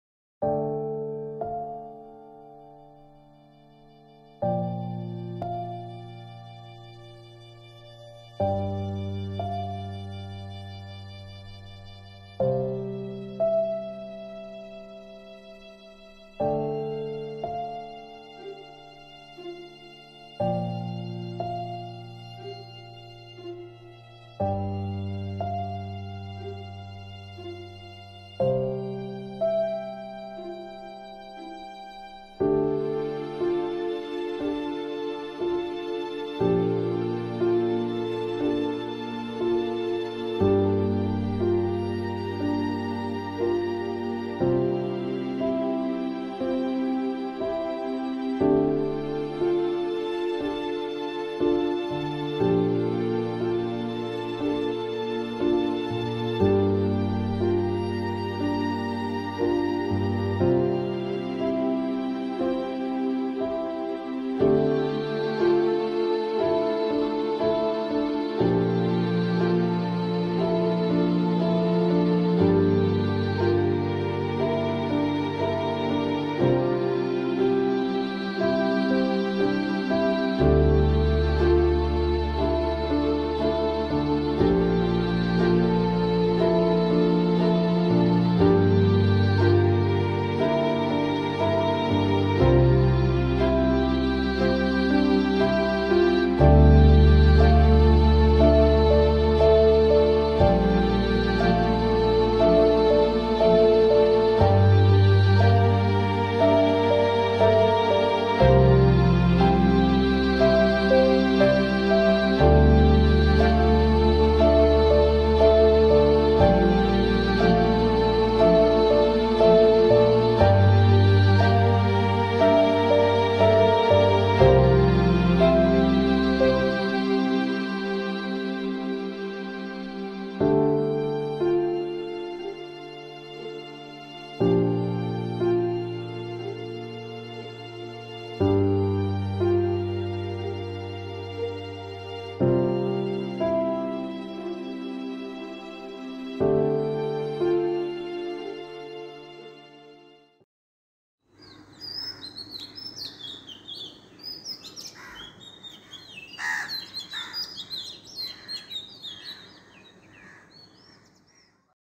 Silence - City in lockdown || A cinematic b roll
This film was shot on Canon 200D, 23 April morning. Because of the world wide pandemic, Rajshahi is under a lockdown as well as the other cities of Bangladesh . The city is silent, the roads are blank. We pray to the All mighty that everything will come back to normal, the sun will shine on us again.